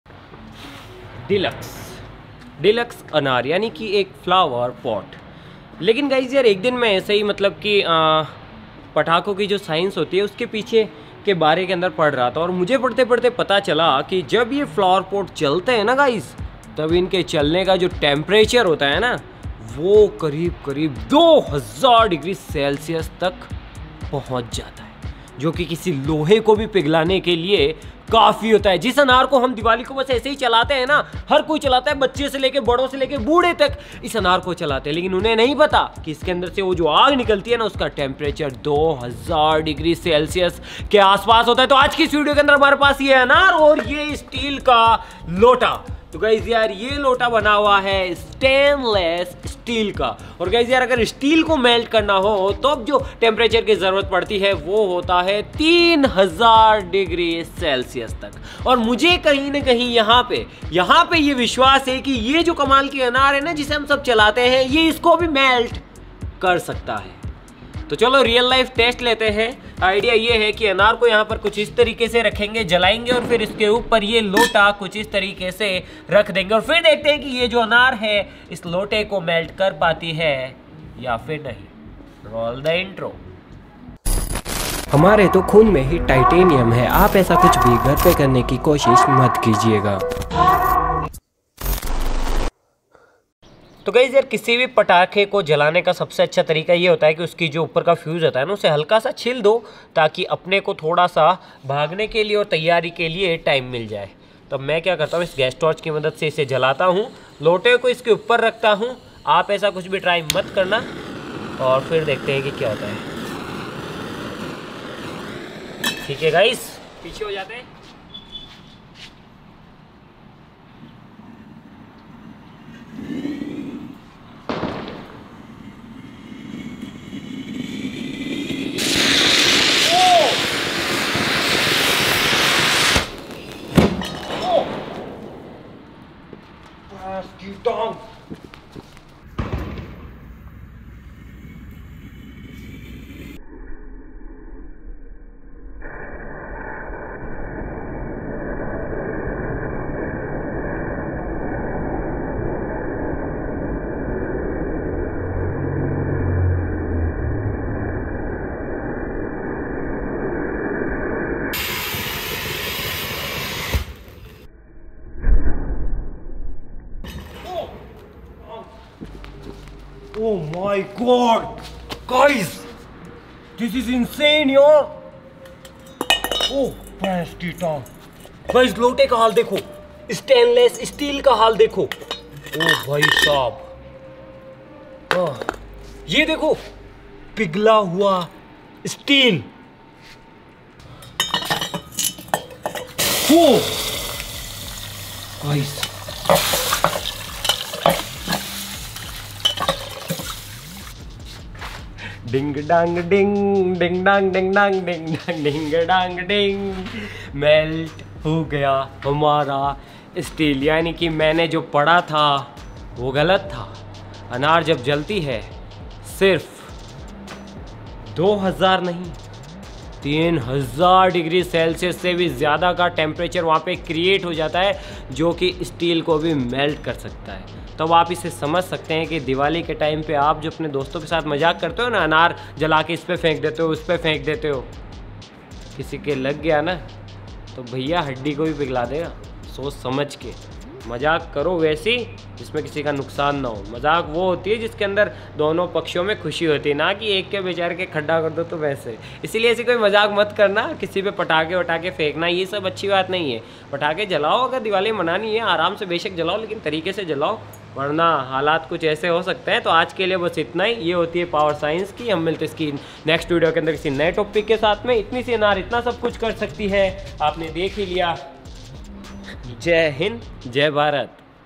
क्या लोटा बच पायेगा ? Diwali Anar Vs Lota...3000°C 🔥
Experiment. Power of science.

English + Tamil + Telugu + Hindi + Kannada...This Video Is Available In Multiple Languages Please Use Audio Track Feature.